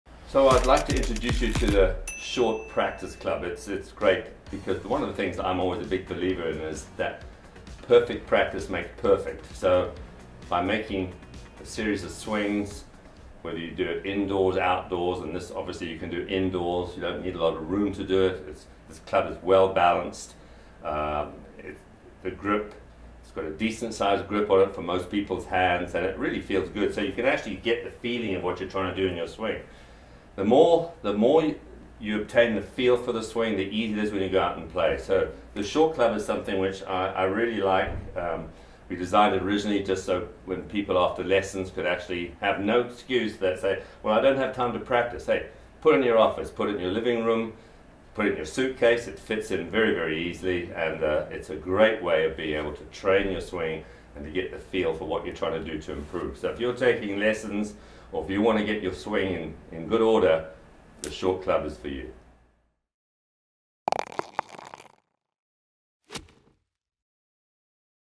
The Leadbetter Short Club
Develop muscle memory and build a repetitive swing motion wherever you are! The Short Club fits in a suitcase and is the perfect way to practice while traveling or confined at home or the office.